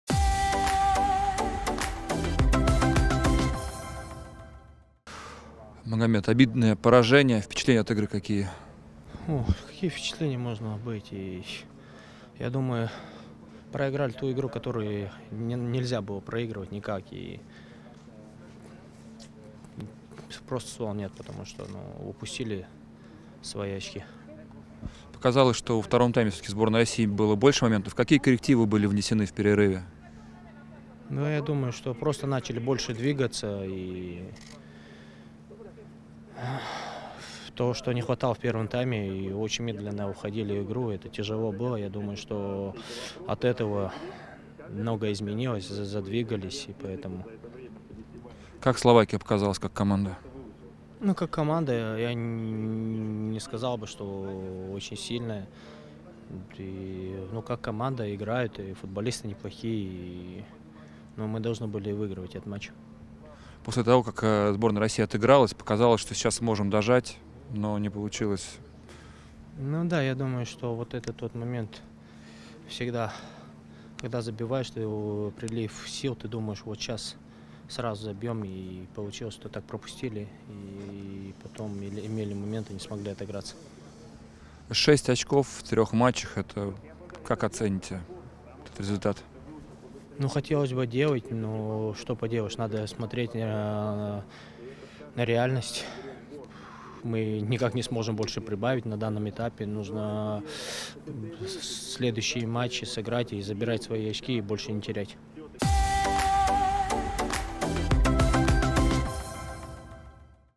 Магомед Оздоев: "После того, как сравняли счет, очень хотели забить ещё"
Полузащитник сборной России подвел итоги матча против Словакии.